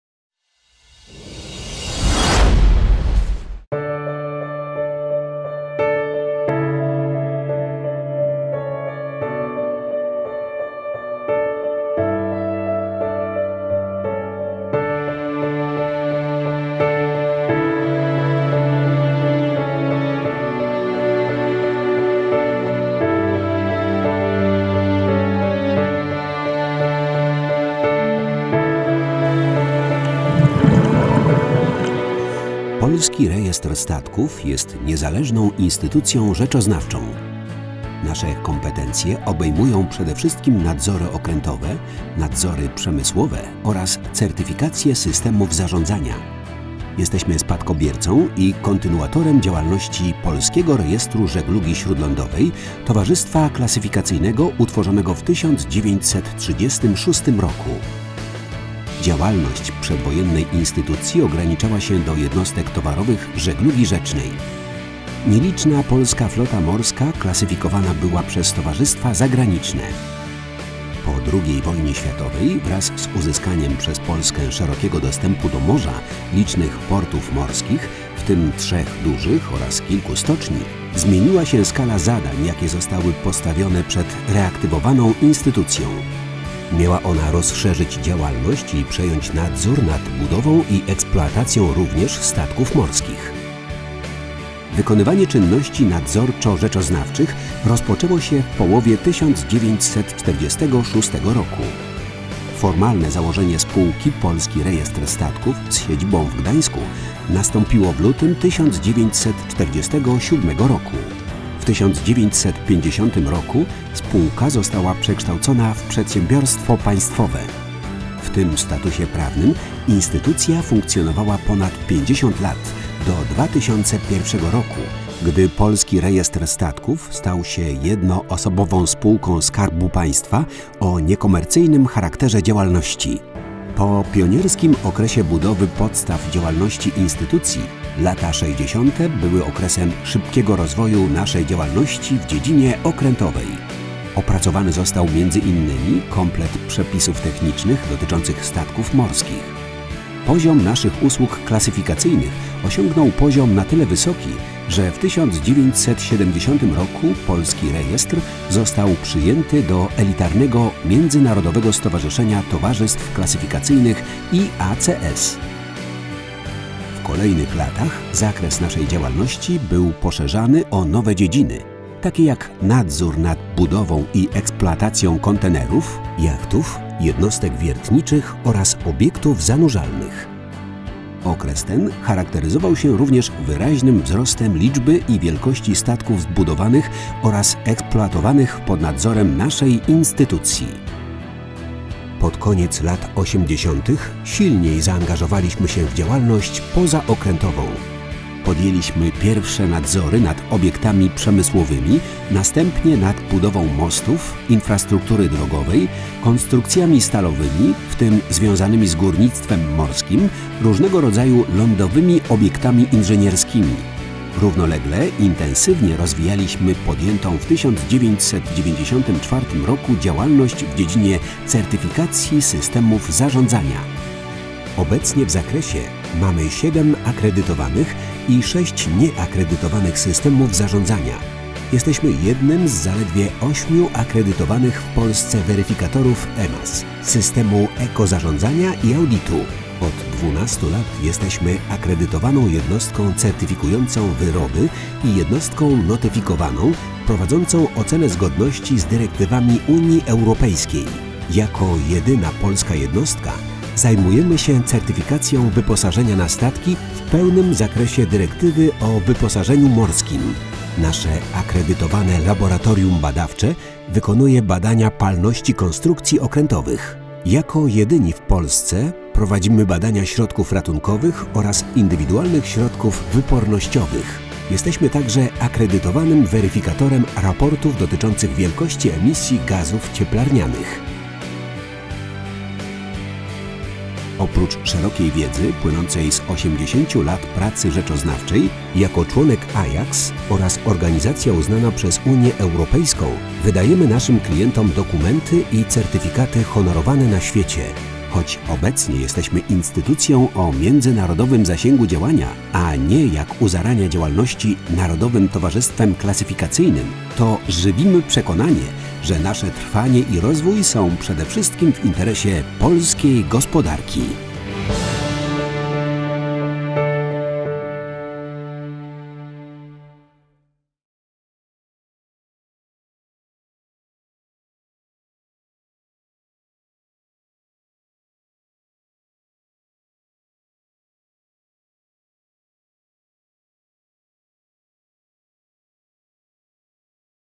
80-lecie Polskiego Rejestru Statków
Polski Rejestr Statków jest niezależną instytucją rzeczoznawczą rozpoznawalną w najdalszych rejonach świata. Jest członkiem IACS i organizacją uznaną przez Unię Europejską. Jubileusz jest znakomitą okazją by przypomnieć najważniejsze fakty z jej historii.